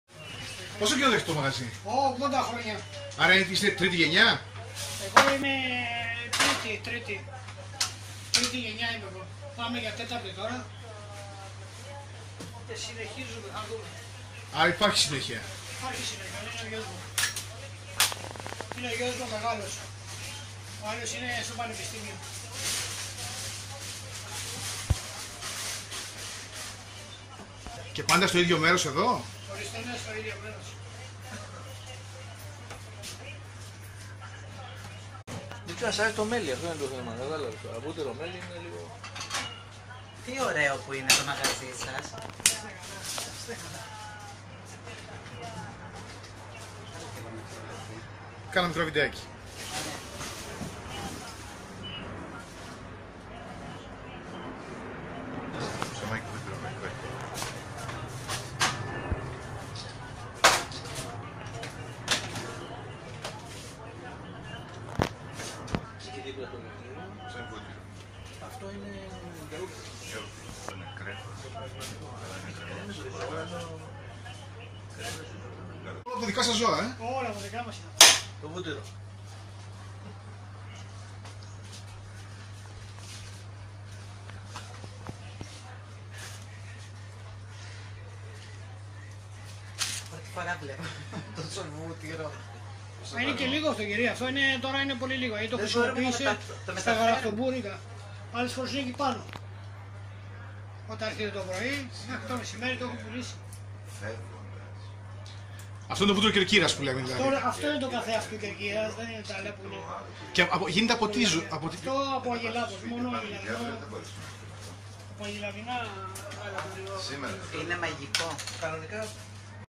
Το ιστορικό γαλακτοπωλείο της Κέρκυρας, της οικογένειας Αλέξη
Στο γαλακτοπωλείο αυτό στα καντούνια της παλιάς πόλης της Κέρκυρας, η οικογένεια Αλέξη, φτιάχνει το βούτυρο Κερκύρας, εδώ και 80 χρόνια